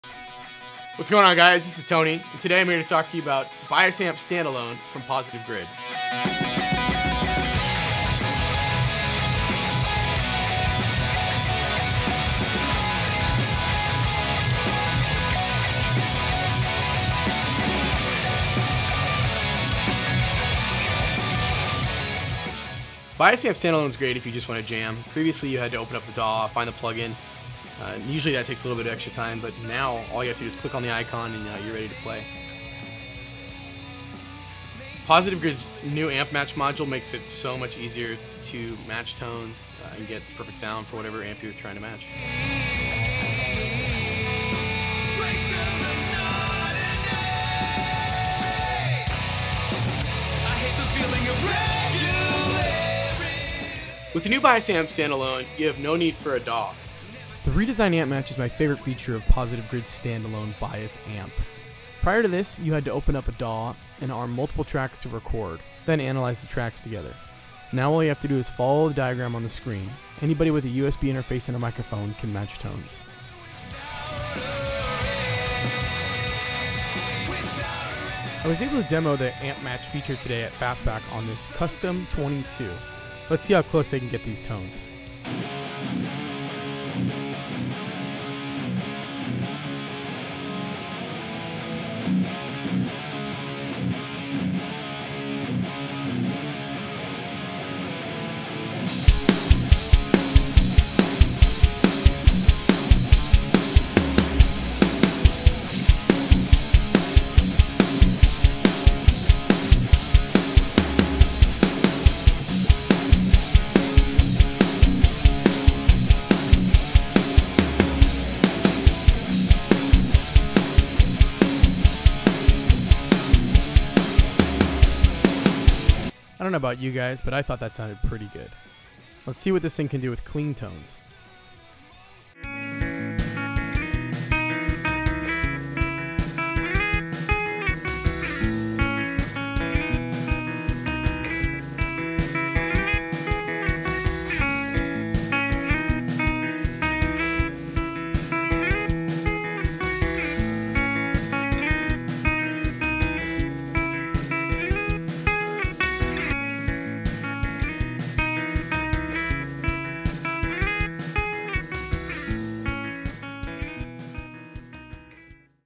Introducing BIAS Amp Standalone
BIAS Amp is now available as a standalone app for Mac/PC - no more DAW needed! New features include a redesigned amp match module without complex DAW audio routing.